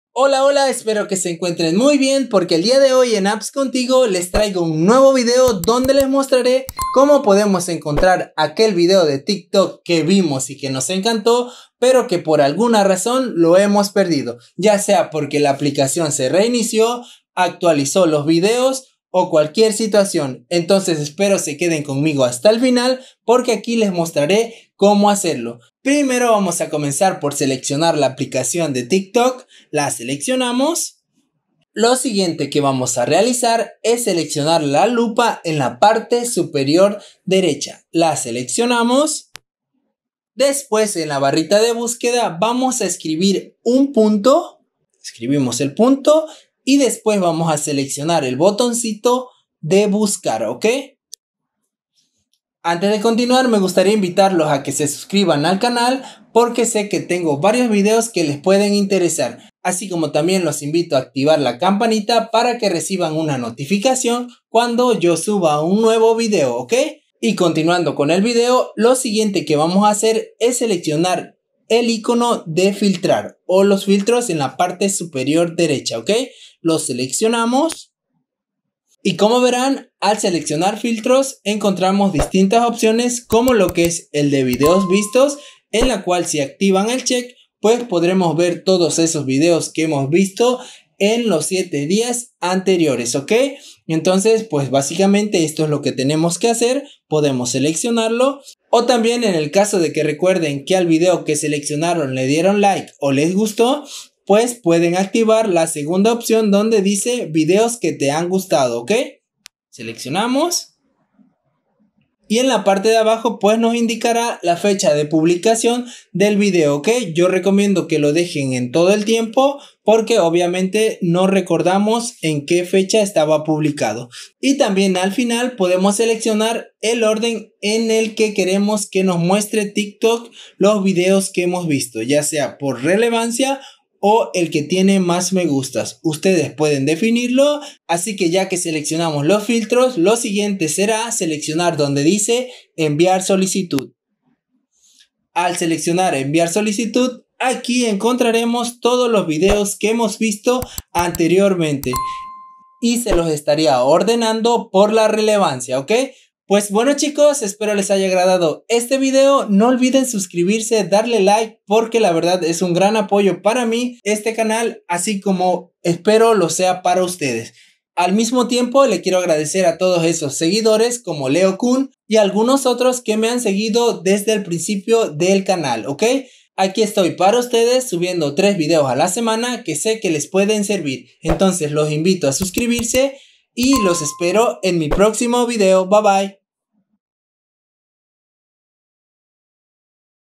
😊😱COMO ENCONTRAR UN VIDEO QUE VI EN TIK TOK😉
😉😱 Hola a todos el día de hoy les mostrare como podemos encontrar el ultimo video que vi en tik tok , así es si por alguna razón perdiste un video que te encanto en tik tok y nos sabes como ver los videos vistos en tik tok , pues aquí te muestro como podemos encontrarlo aunque no le hayas dado like , ya verán que es muy sencillo realizarlo solo deben seguir los pasos que les muestro a continuación y listo podrán encontrar todos esos tik tok que vimos anteriormente , espero les agrade este video no olviden suscribirse darle para mas contenido como este .